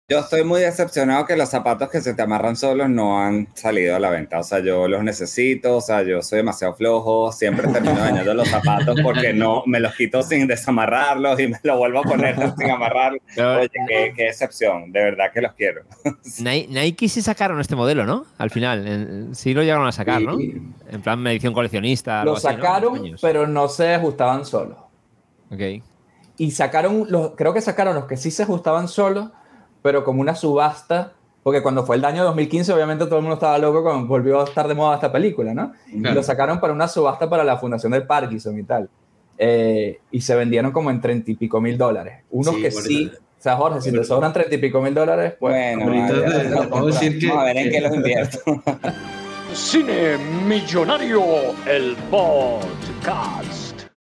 ¿Decepcionantes sneakers? - Volver al futuro 2 - Ep. 220
Este clip pertenece al episodio 220 que puedes encontrar en Cine Millonario - El Podcast.

¡Nos acompaña el GRAN Jorge Roig, periodista y presentador de Sin Spoiler en Venevision Plus y experto en viajes en el tiempo y futuros alternativos!